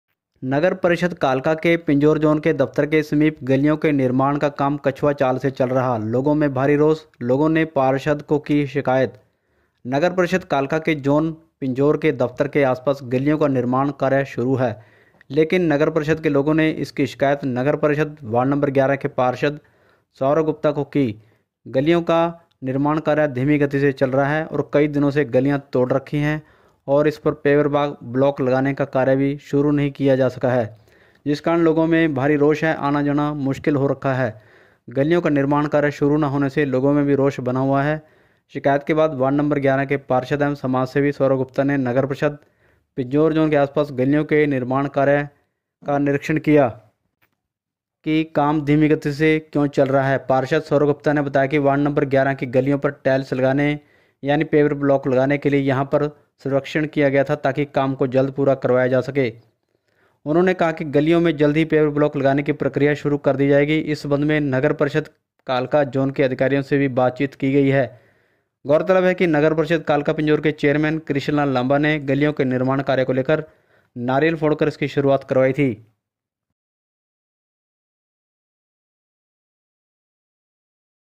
pinjore वार्ड 11 में गलियों का निर्माण धीमा, लोगों की शिकायत।
वार्ड 11 में गलियों का निर्माण धीमा, लोगों की शिकायत आई, बाद में पार्षद सौरभ गुप्ता ने किया निरीक्षण।